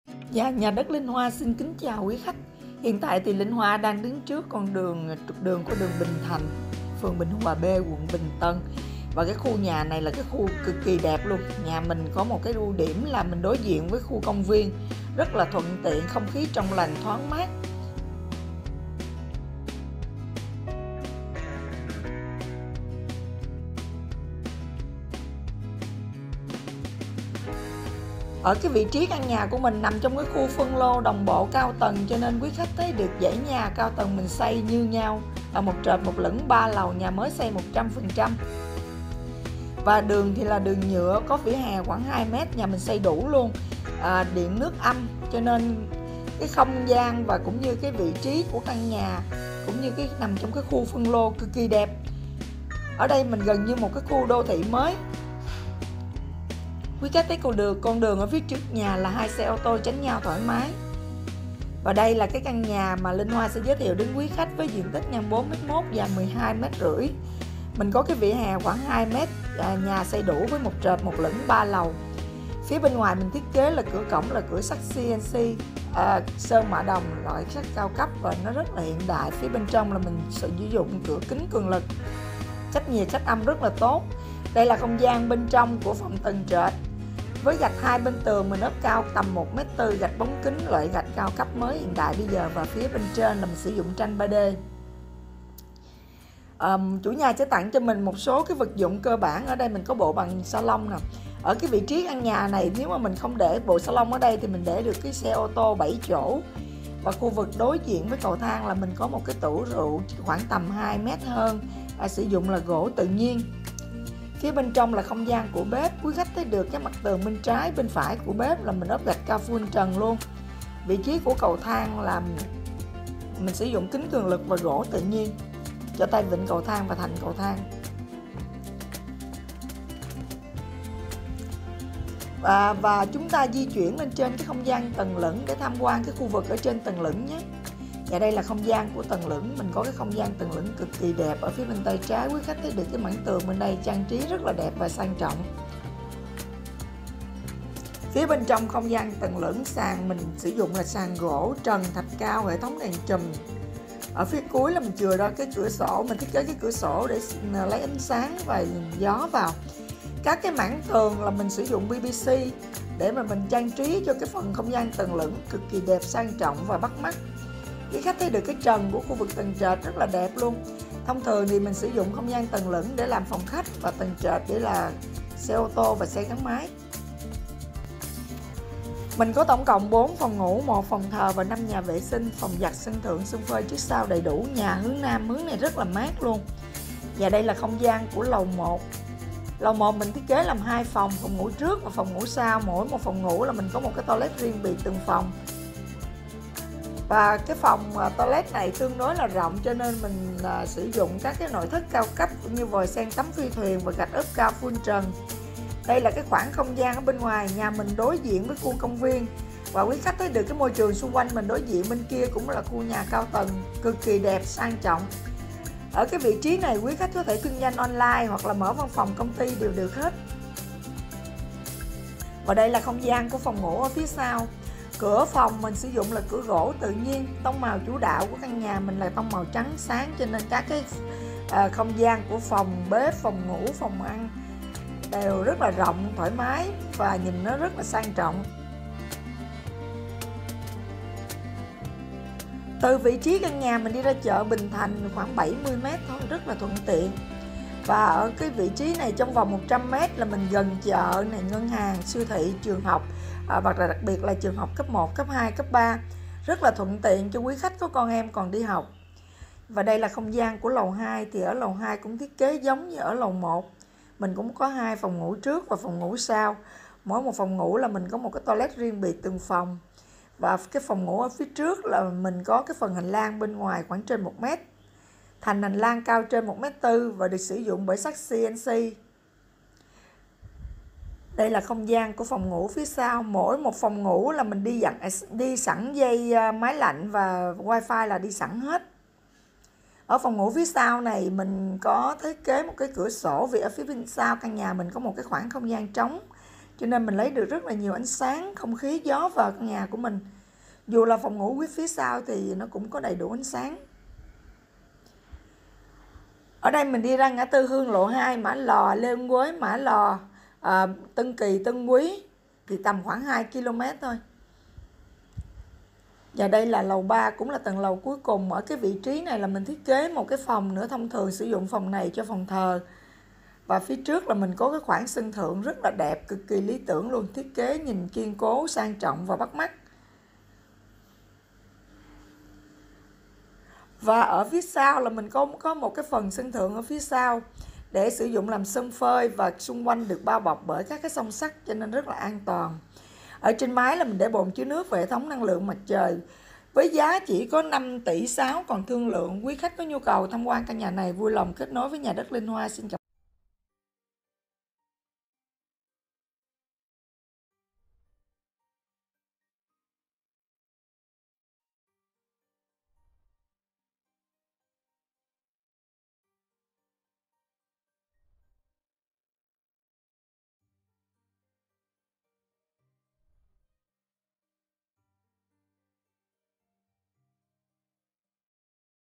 Khu nhà đồng bộ cao cấp quận Bình Tân đối diện công viên trên 5 tỷ
Nhà ở quận bình tân bán giá rẻ trên 5 tỷ cao cấp đầy đủ nội thất. Đối diện công viên, khu an nình dân trí thoáng mát tiện ích đầy đủ
Nha dat Linh Hoa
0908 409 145 zalo  hoac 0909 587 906

nhà bình tân,
nhà đất bình tân,
bán nhà quận bình tân,
bán nhà bình tân,
nha dat binh tan,